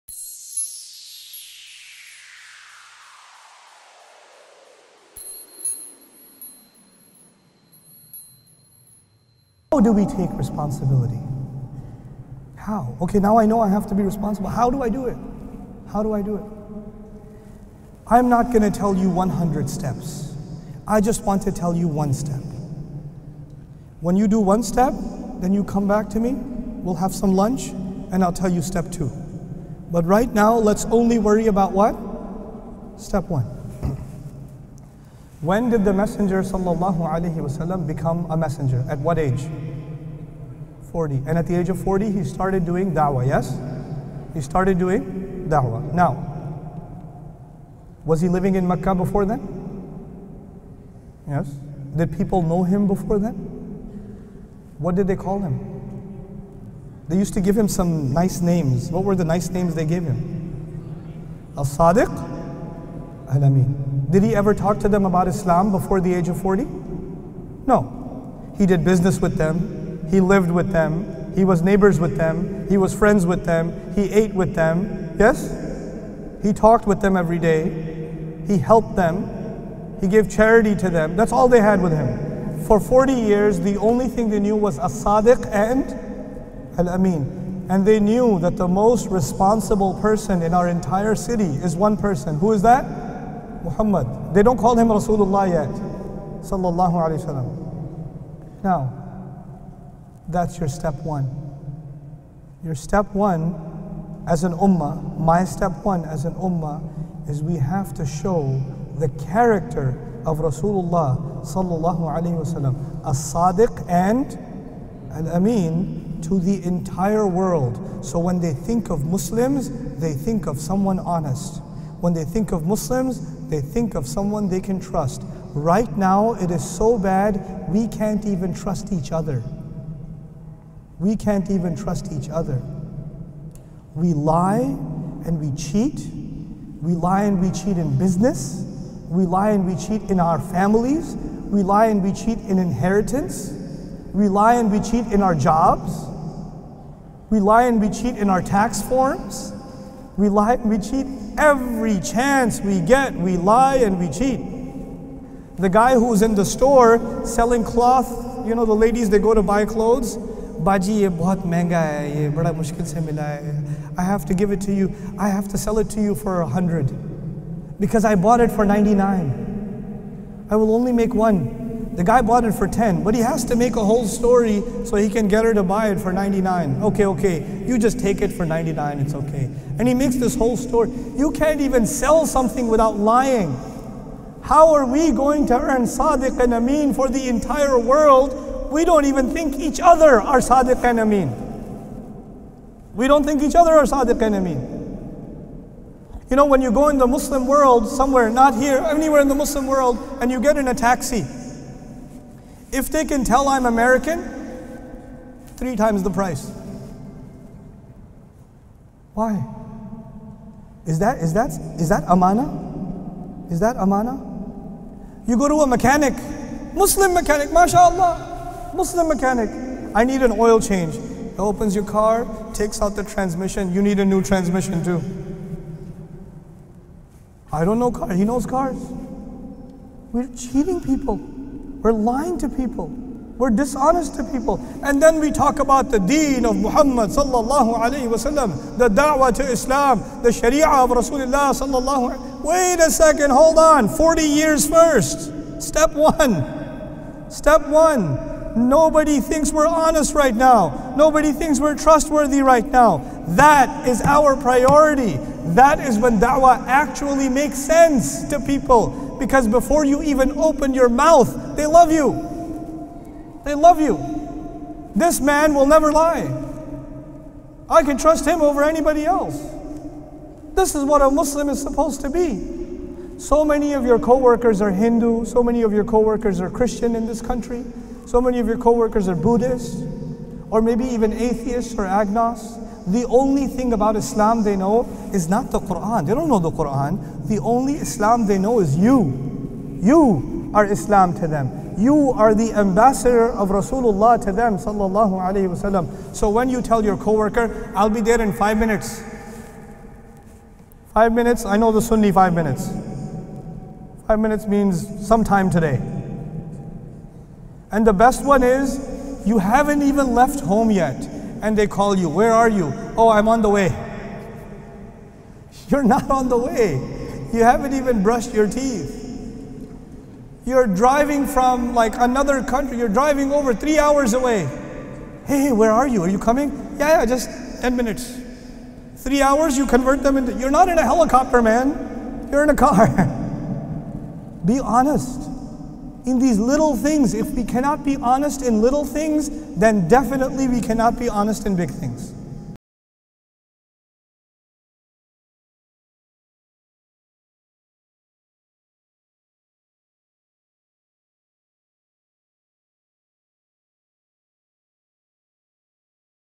The First Step to being a Muslim - Nouman Ali Khan
You are the Ambassador of Islam, are you doing a Good Job?, Do people around you perceive you as an Honest & Trustworthy person? Before people will ever read about Islam they will try to understand it by looking at the behavior of Muslims, are we doing a good job?. Action speaks louder than words.

🔁 Share and Help us spread the Message of Ustadh Nouman Ali Khan - @bayyinah